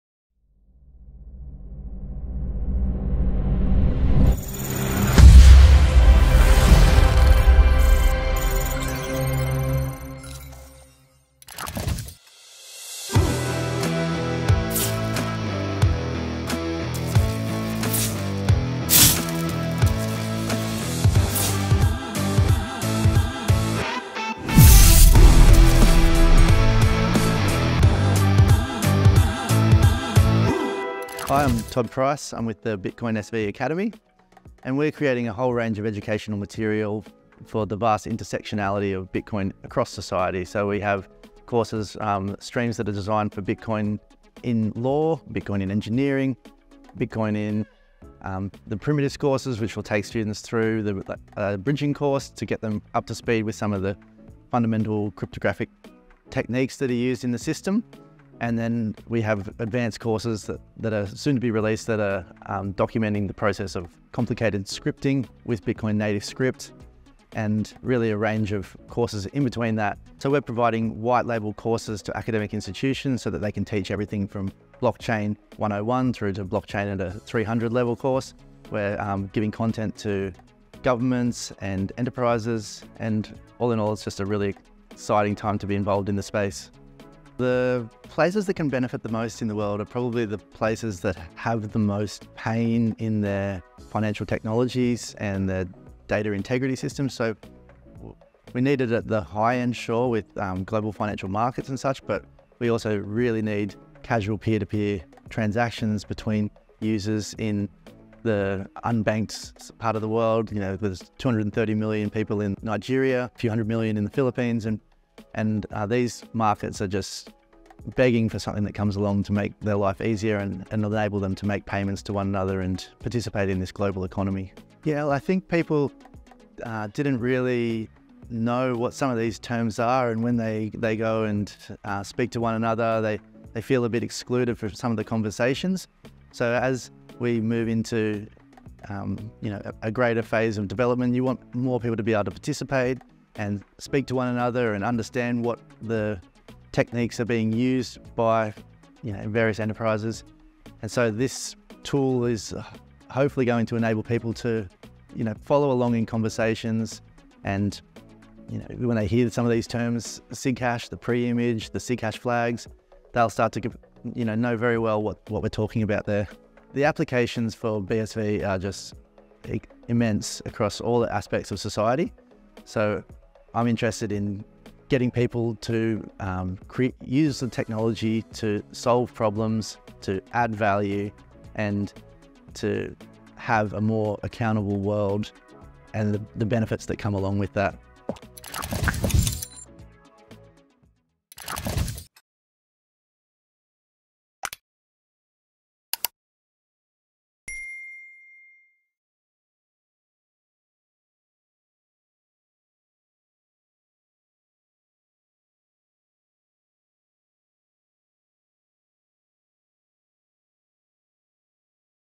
In the Spotlight: Todd Price on the BSV Academy
As a relatively nascent technology, it is important to educate the wider public about the blockchain and exactly how it works.

The BSV Academy is an online education platform focusing on blockchain and Bitcoin which offers academia-quality, university-style courses and learning materials categorised under Bitcoin Theory, Bitcoin Development, Bitcoin Infrastructure and Bitcoin Enterprise, in addition to several short courses covering specific concepts and content in more detail.